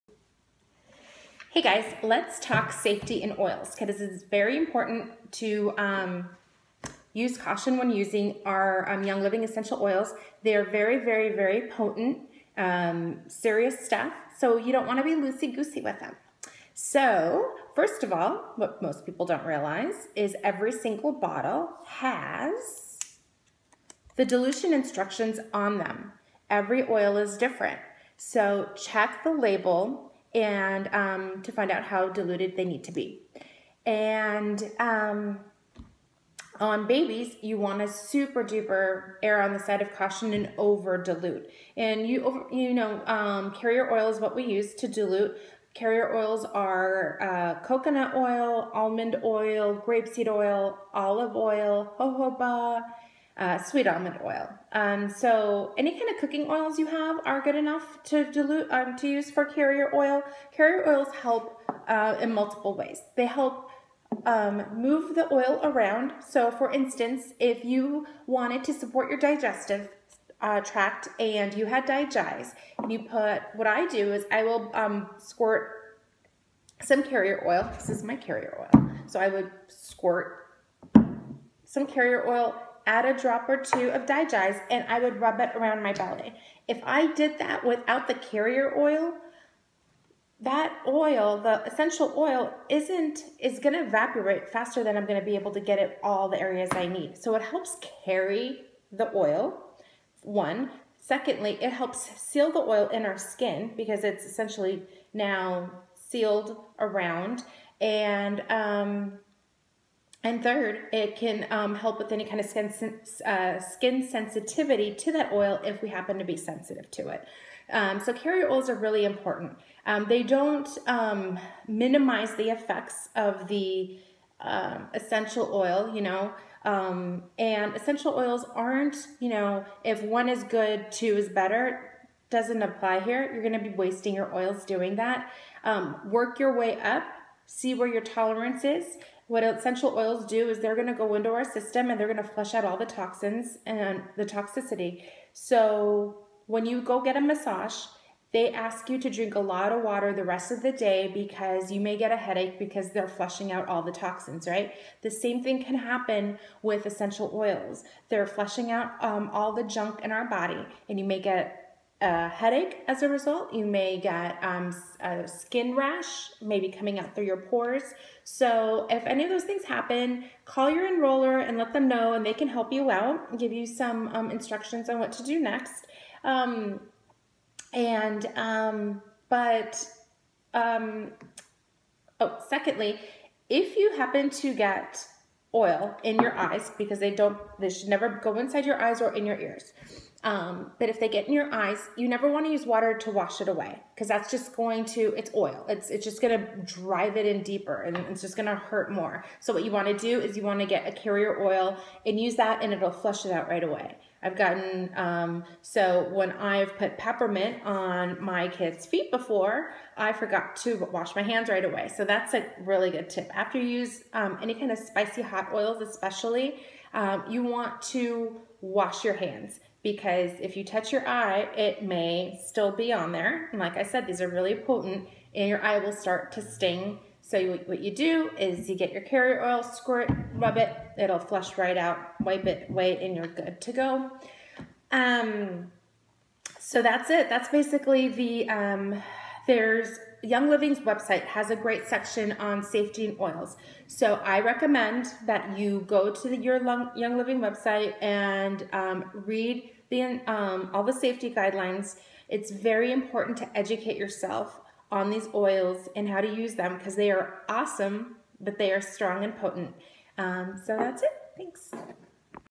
Oil Safety - Young Living Essential Oils
How to use Young Living Essential oils safely.